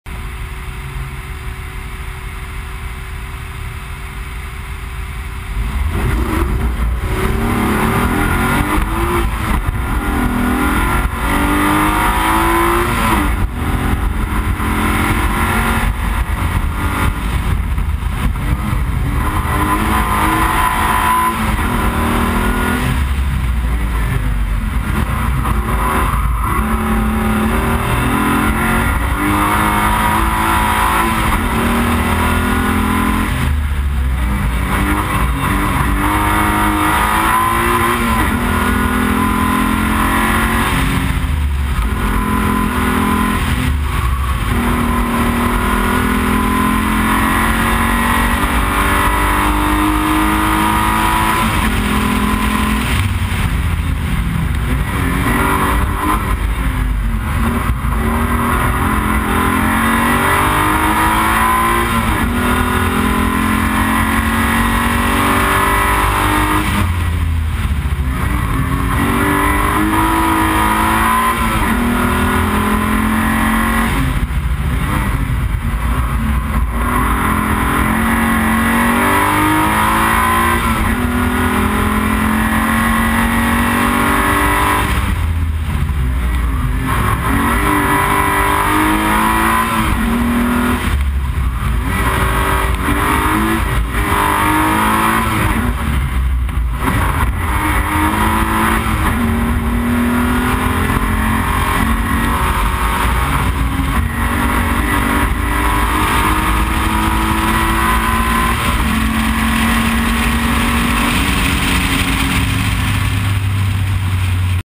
KP Telnice 11.8.2012 - Dan Mihalík - Alfa Romeo 156 GTA
time: 1:43:73s